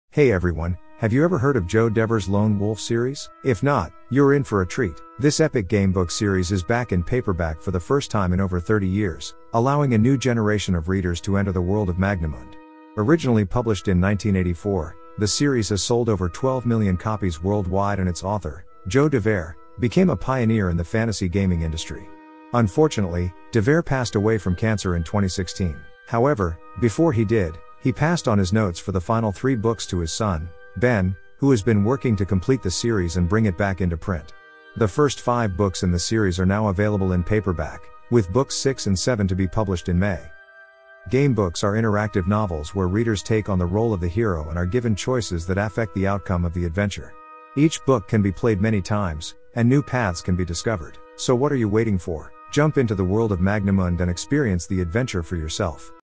Joe Dever's Lone Wolf Gamebook Series Returns in Paperback After 30 Years
Joe Dever's epic gamebook series, Lone Wolf, is back in paperback for the first time in over 30 years, allowing a new generation of readers to enter the world of Magnamund. Originally published in 1984, the series has sold over 12 million copies worldwide, and its author Joe Dever became a pioneer in the fantasy gaming industry. Dever passed away from cancer in 2016, but before he did, he passed on his notes for the final three books to his son, Ben, who has been working to complete the series and bring it back into print.